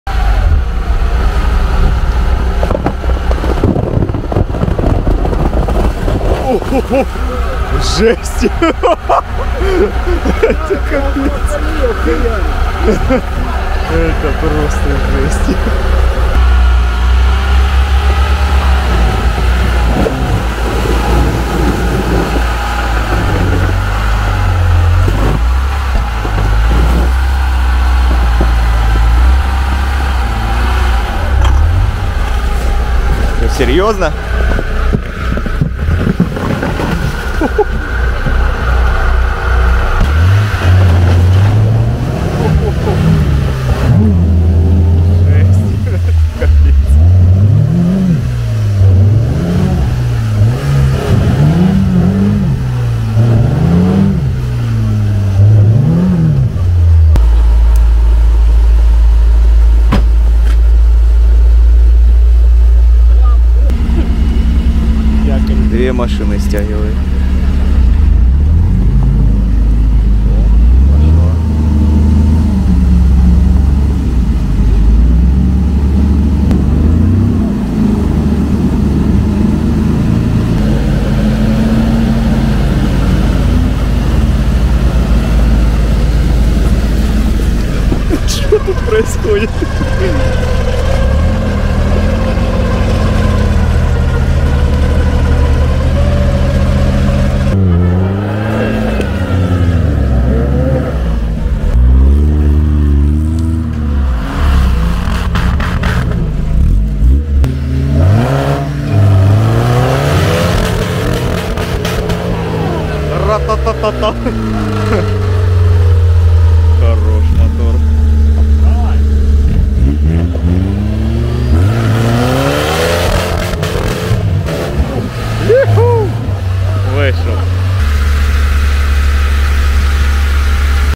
Как УАЗ УШЁЛ под ЗЕМЛЮ
Характеристики машины:

- УАЗ 469:
Мотор ЗМЗ 409 2.7 МКПП
33” Silverstone MT-117 Xtreme
Военные мосты, самоблоки перед/зад